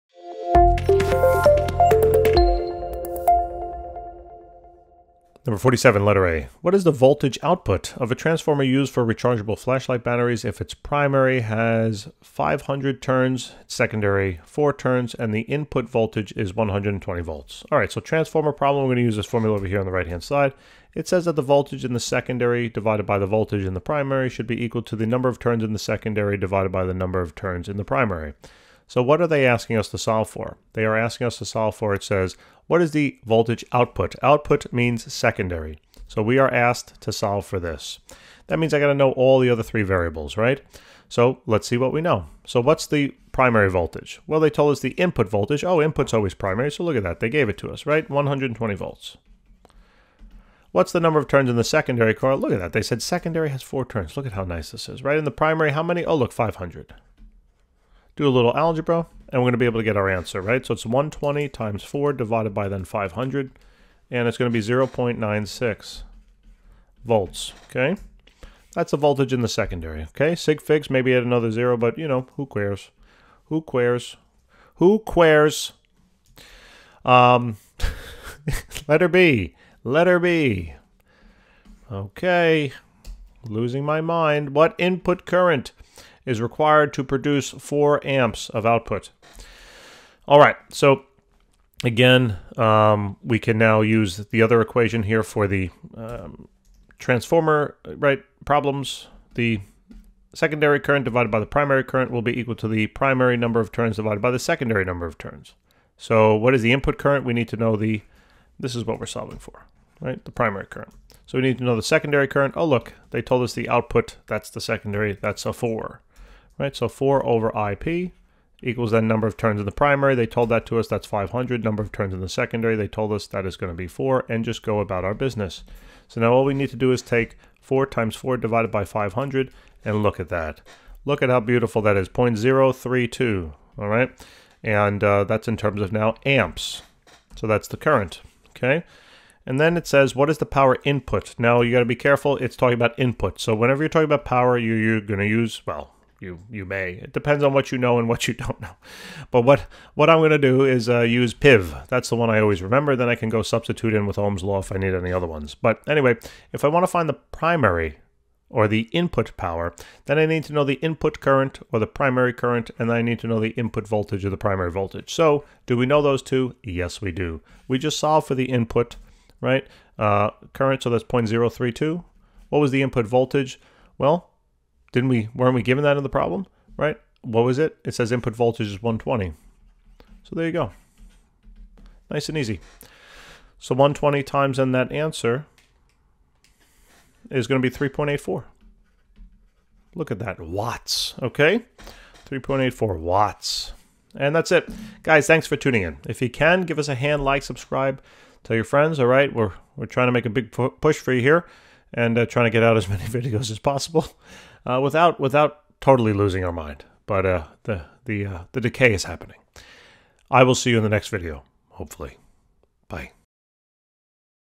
23.47 | What is the voltage output of a transformer used for rechargeable flashlight batteries, if
(a) What is the voltage output of a transformer used for rechargeable flashlight batteries, if its primary has 500 turns, its secondary 4 turns, and the input voltage is 120 V? (b) What input current is required to produce a 4.00 A output? (c) What is the power input?